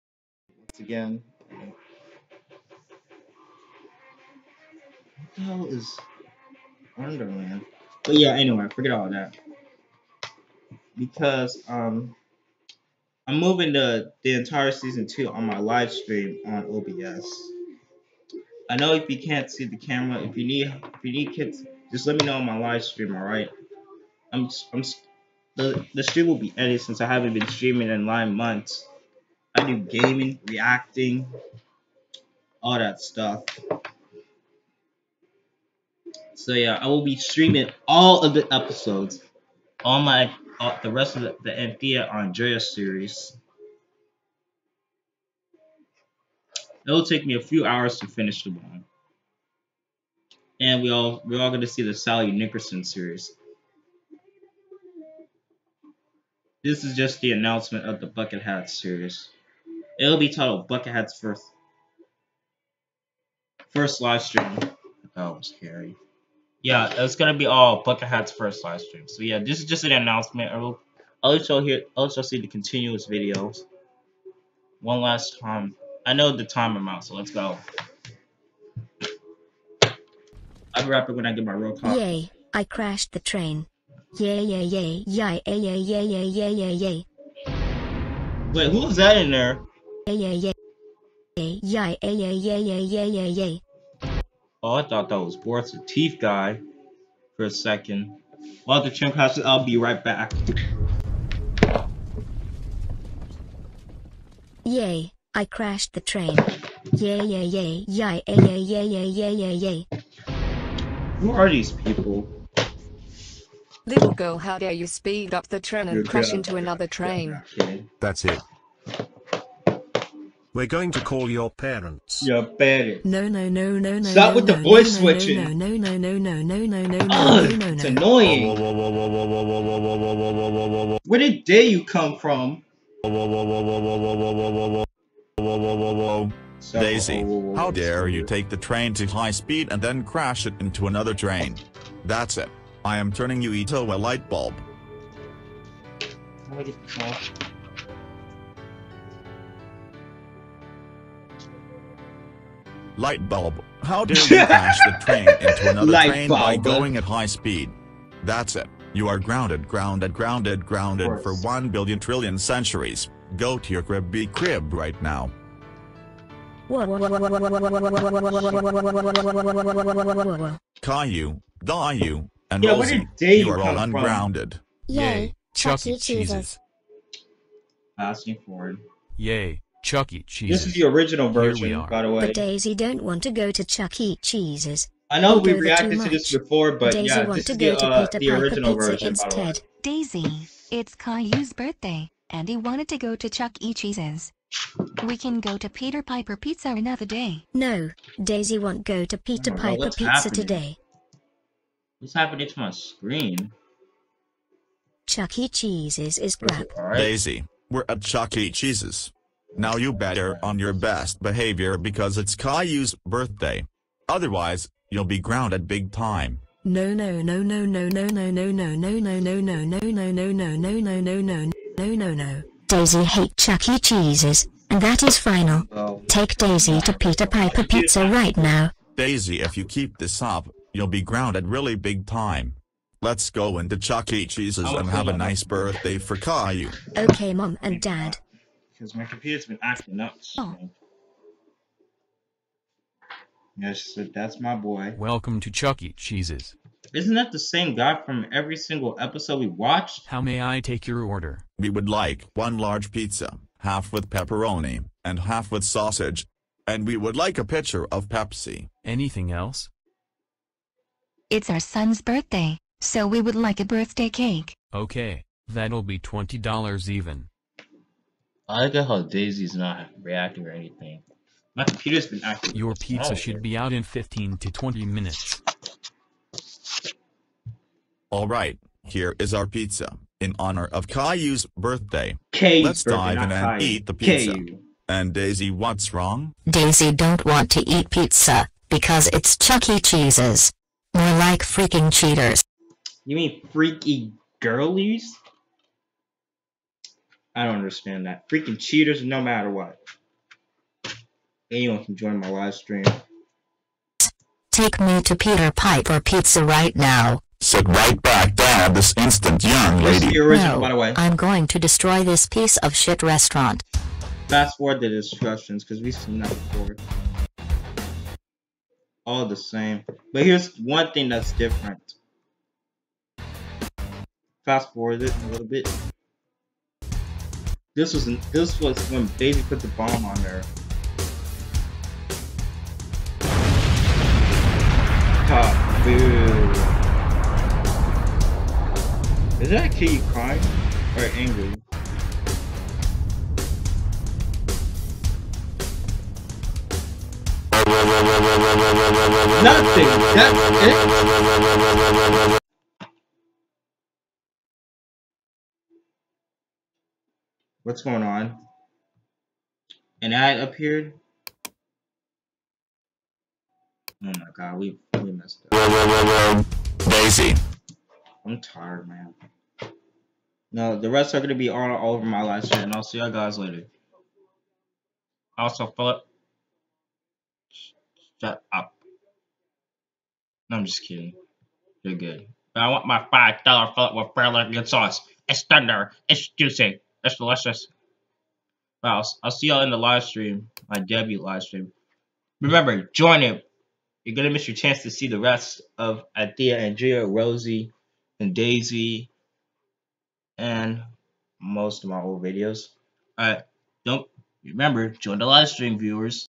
Bucket hat Maurice reacts #29: Rosie and daisy gets season 2 part 3
wow daisy you got serious issues. Bucket hat Maurice (both taken May 31st,2022) reacts to Rosie and daisy getting ground part 3. Is it good or not?